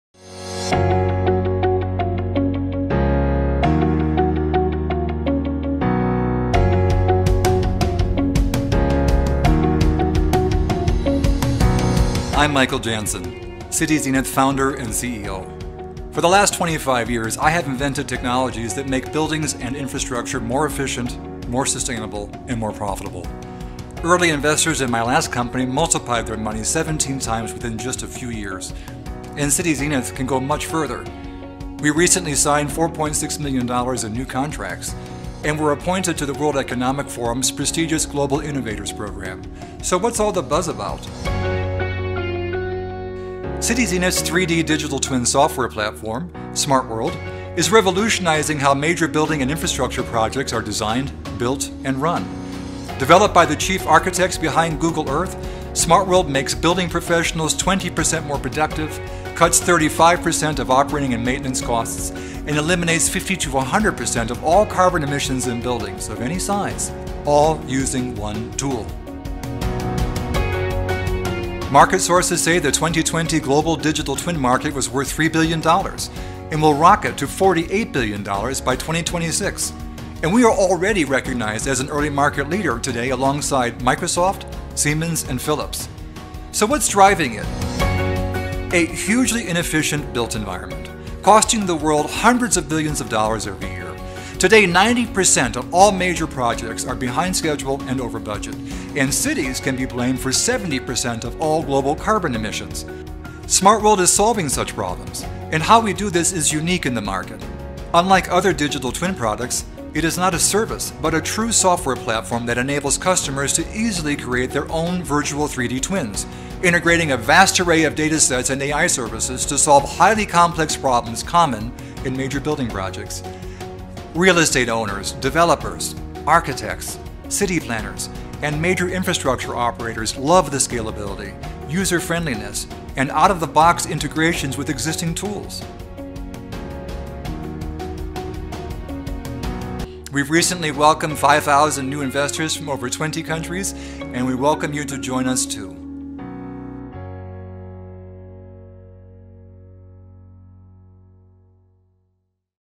Cityzenith Company Overview - 2022
39% of global carbon emissions come from the buildings in our cities.

Less than 2% of the USA's 6 million office buildings are green.

What if we could help buildings, infrastructure and cities reach net-zero faster, without risk and at a fraction of the cost?

At Cityzenith, that's what we do.

to find out more.